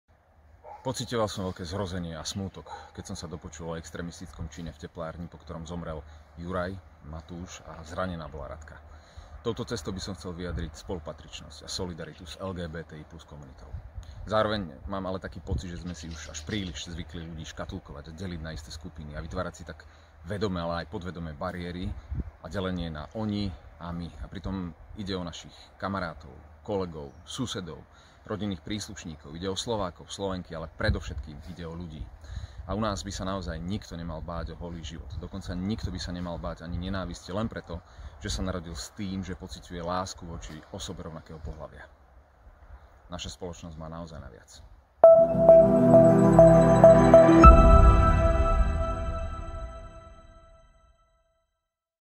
Roman Juraško | Nie je nám to jedno 💔 smesvami 🏳️‍🌈
„Mám pocit, že sme si príliš zvykli ľudí škatuľkovať a deliť na isté skupiny, čím si vytvárame vedome a podvedome bariéry. Delíme sa na my a oni, pritom ide o našich kamarátov, rodinných príslušníkov a kolegov a predovšetkým ide o ľudí,“ hovorí moderátor Roman Juraško, ktorý veľmi citlivo vnímal nedávne tragické udalosti a nenávistné prejavy voči komunite LGBTI+ 🏳️‍🌈

Nie je nám to jedno 💔 Známe osobnosti sa rozhodli, že nebudú mlčať a dali v spolupráci s nami jasne najavo svoj názor na udalosti posledných dní. Postupne zverejníme ich silné odkazy plné podpory. Sme s vami 🏳️‍🌈